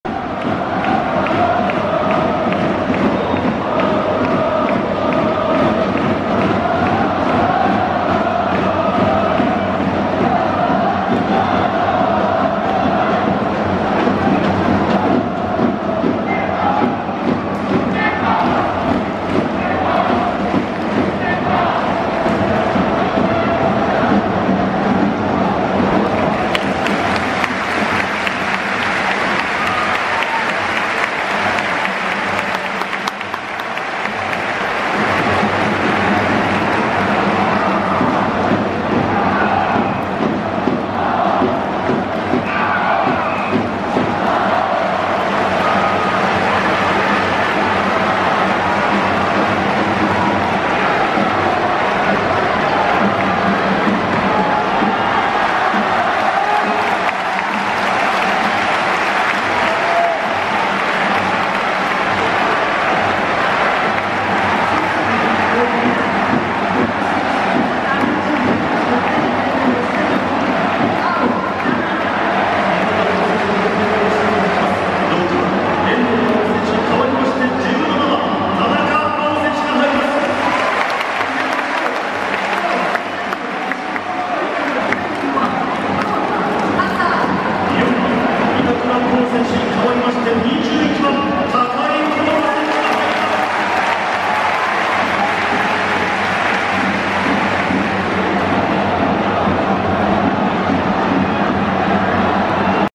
髙井幸大、フロンターレの先輩板倉滉と途中出場でA代表デビューの瞬間。2024/09/05ワールドカップ最終予選 日本代表ー中国代表 埼玉スタジアム
2024/09/05ワールドカップ最終予選　日本代表ー中国代表　埼玉スタジアム